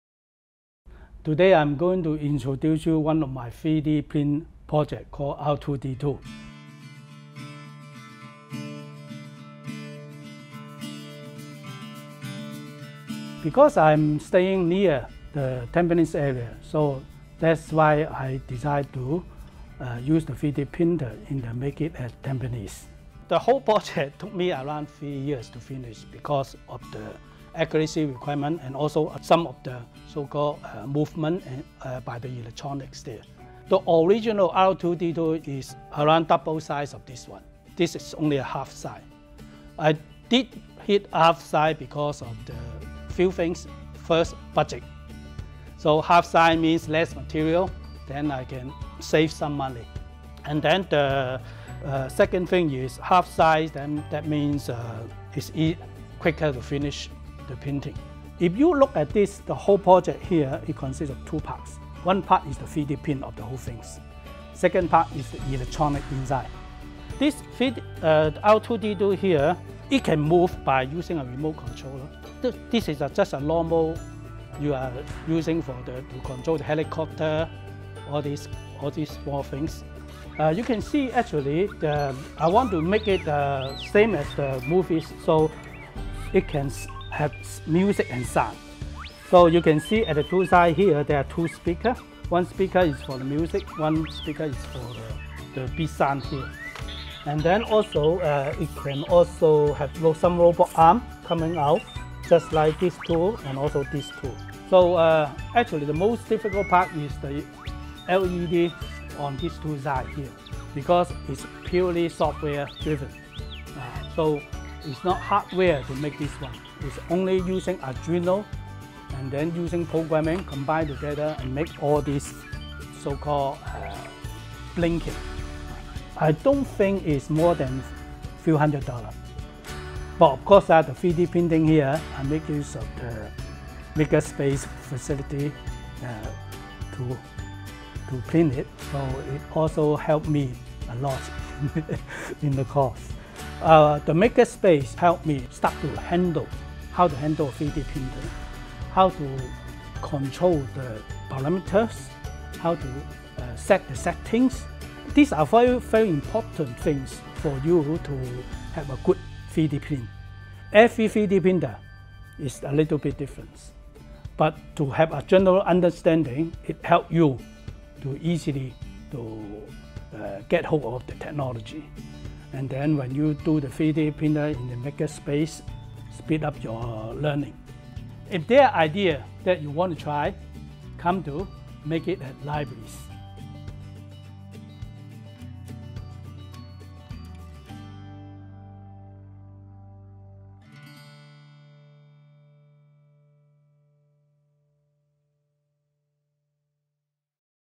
Steve Makes His Own R2-D2 At The Library! | Makers Next Door
Steve Tam, an avid user at MakeIT at Libraries, shows us that with a little spark of creativity, you can bring your favourite movie characters to life! No force-users required.

Learn more about his hard work in creating his own R2-D2 from scratch with 3D printing, coding, and more at MakeIT! 🦾

Timestamps:
00:00 – Meet Steve Tam!
00:13 – What got you into 3D printing?
00:23 – How long did it take you to 3D print the R2-D2?
01:06 – Introducing the R2-D2 features
02:09 – How did MakeIT’s makerspaces help you in this project?
03:02 – Closing